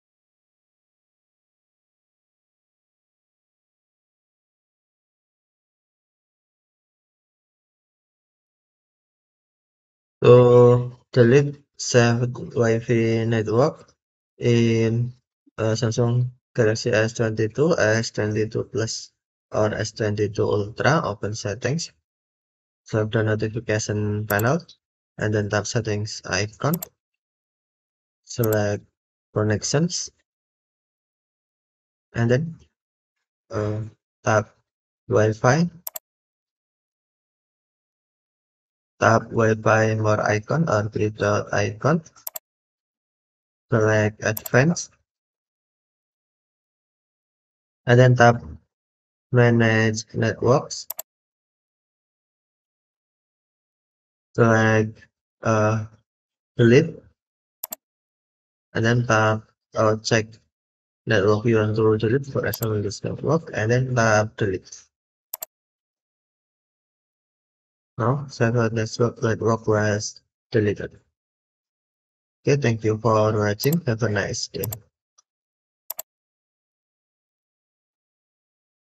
Samsung Galaxy S22 Ultra : How to delete Saved WiFi Network
In this video, you will find the steps on How to delete Saved WiFi Network in Samsung Galaxy S22/S22+/S22 Ultra.  This video uses Samsung Galaxy S22 Ultra SM-S908E/DS International version runs with Android 12